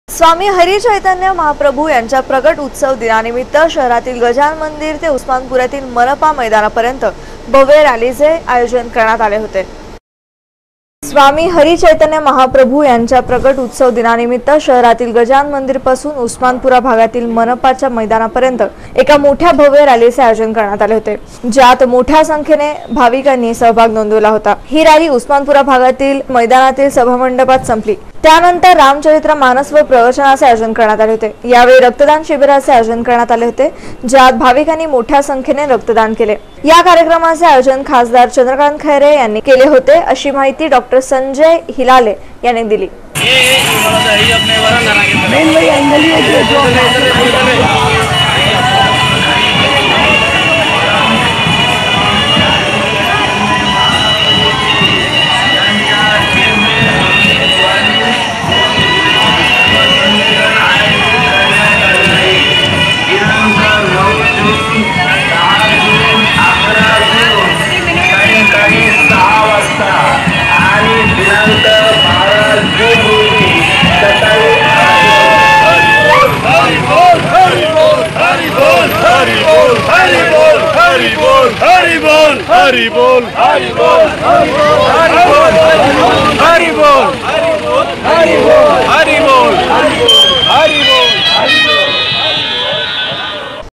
स्वामी हरि चैतन्य महाप्रभु यांच्या प्रकट उत्सव
स्वामी हरि चैतन्य महाप्रभु यांच्या प्रकट उत्सव दिनानिमित्त शहरातील गजानान मंदिर ते उस्मानपुNयातील मनपा मैदानापर्यंत भव्य रॅलेचे आयोजन करण्यात आले होते...